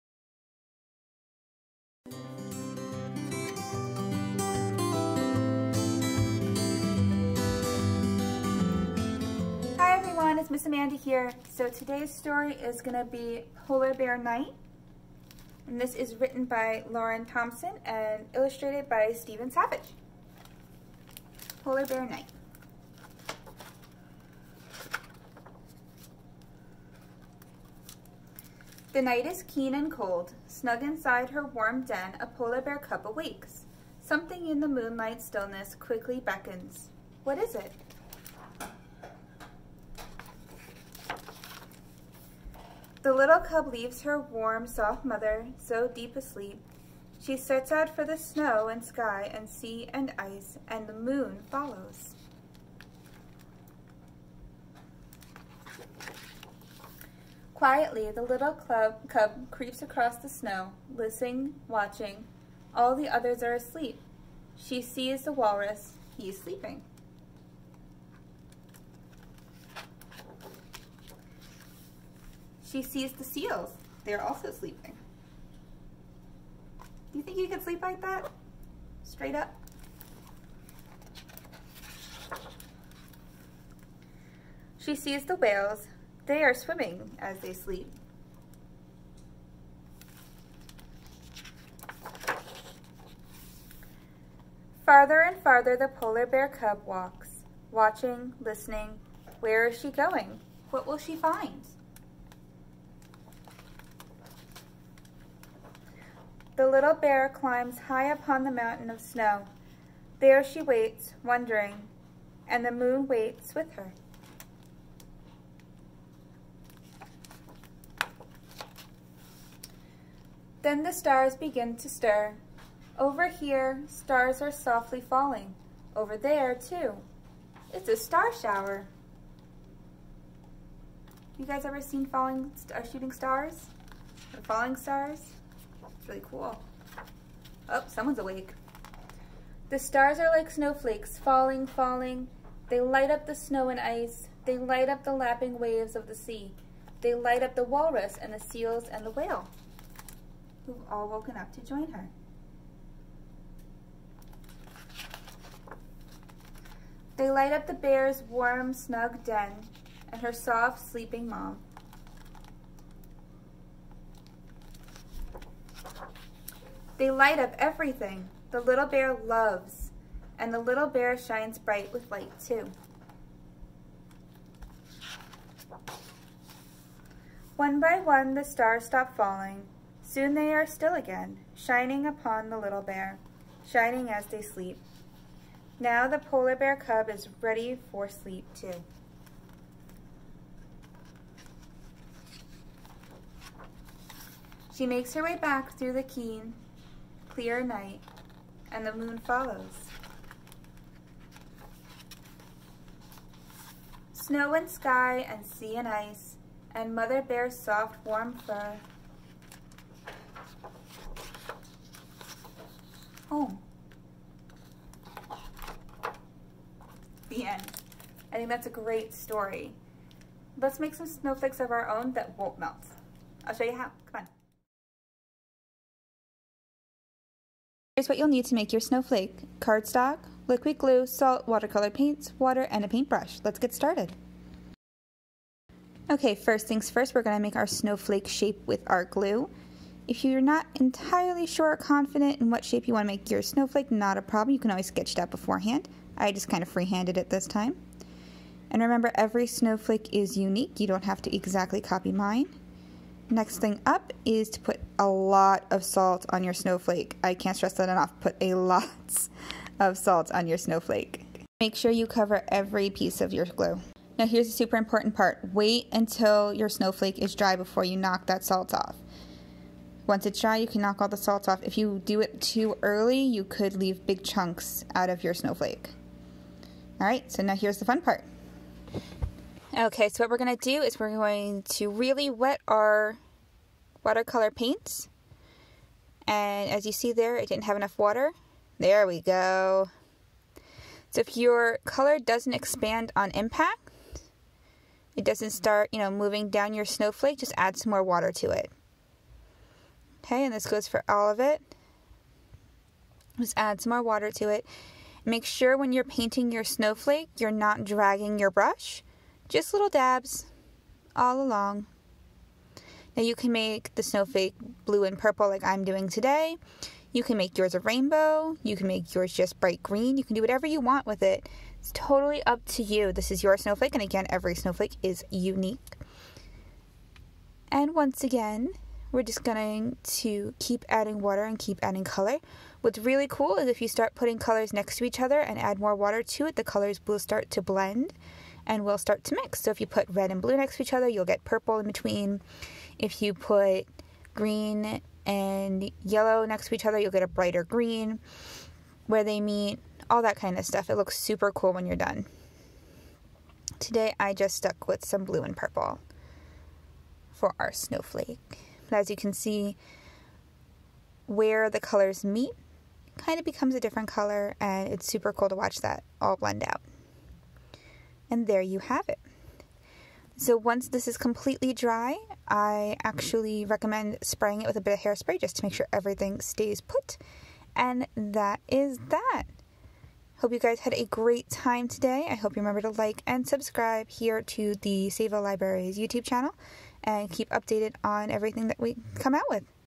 3D Snowflake Painting with Miss Amanda
Watch Miss Amanda show you how to create beautiful, rainbow 3D snowflakes and read a cozy winter story.
Registrants will be emailed a list of materials and a link to watch the video and paint along.